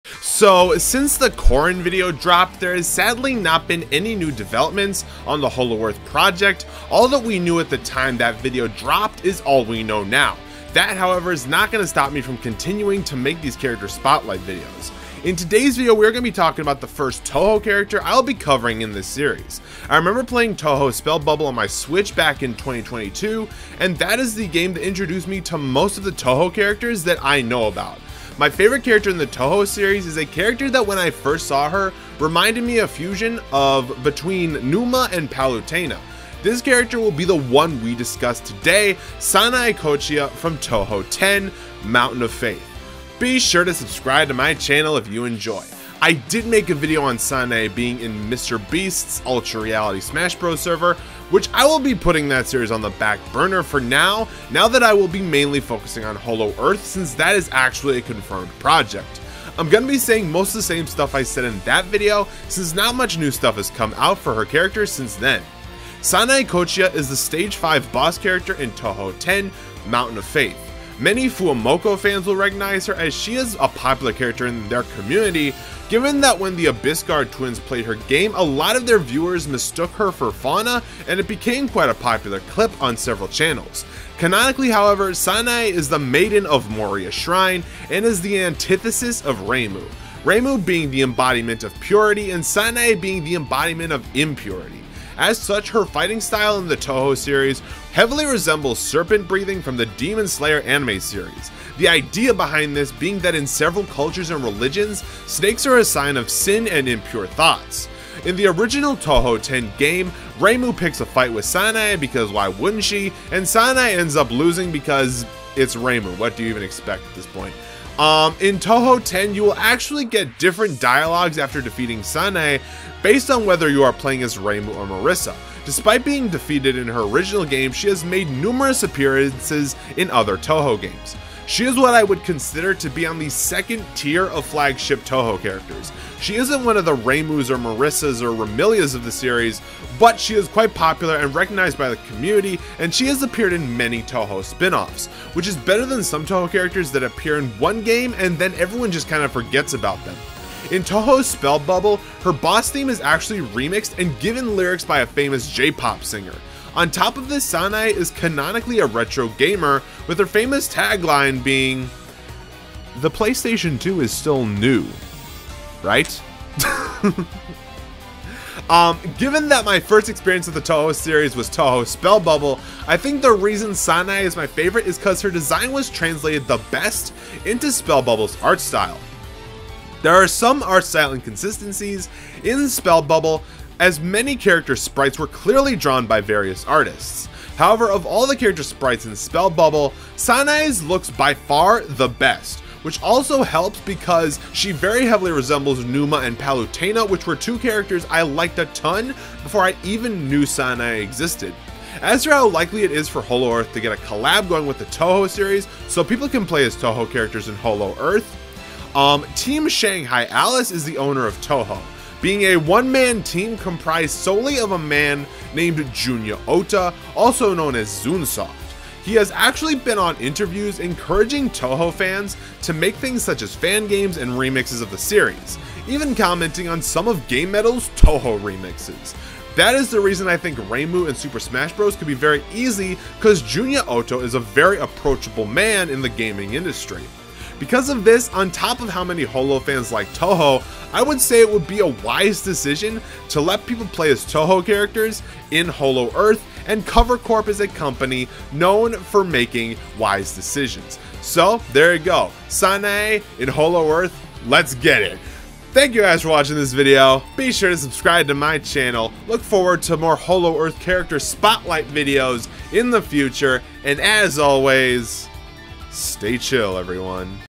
HoloEarth Character Spotlight #3: Sanae (Touhou 10: Mountain of Faith)
A wind priestess with the power of a god, sounds hype if you ask me.

I think it will be easy for Touhou characters to be playable in HoloEarth cuz Zun is very easy to talk to so yeah. Plus a lot of Holo fans like Touhou so it should be a gimme.